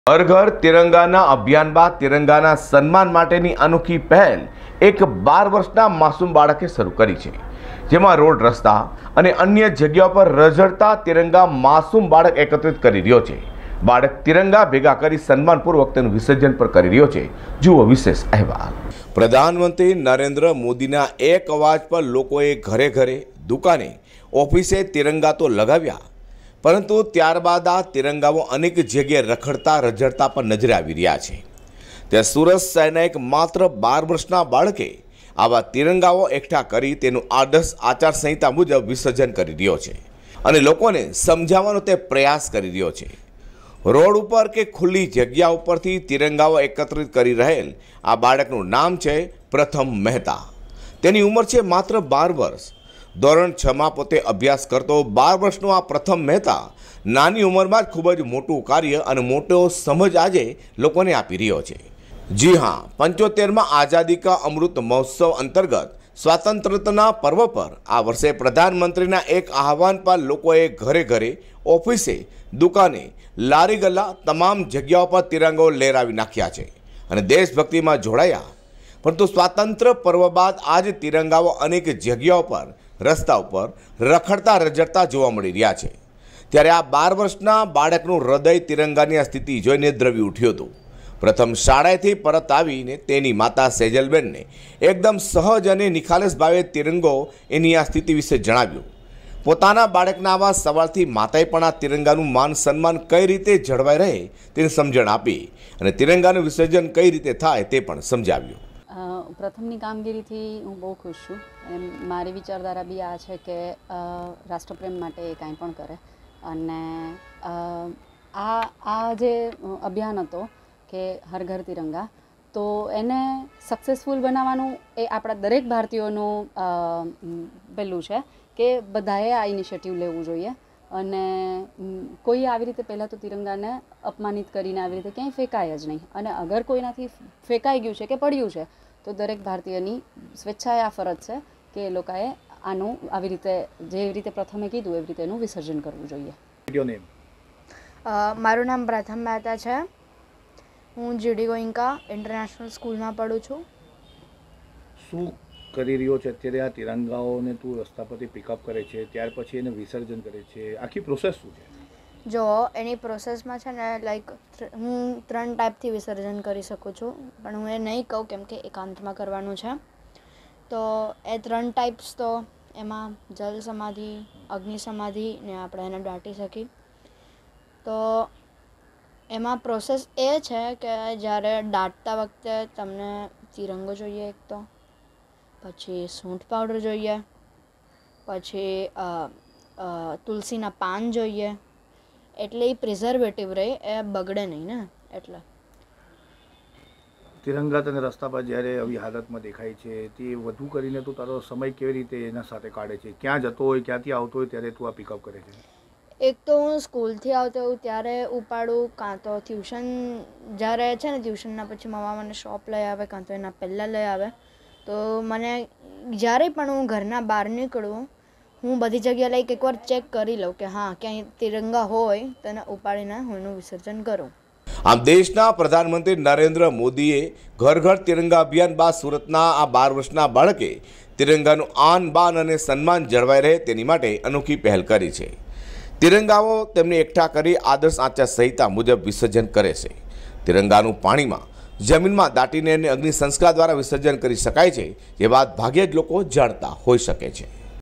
સુરતના 12 વર્ષના બાળકની અનોખી પહેલ@samaymedianewssurat2833
હર ઘર તિરંગા અભિયાન બાદ માસુમ બાળક તિરંગા એકત્ર કરે છે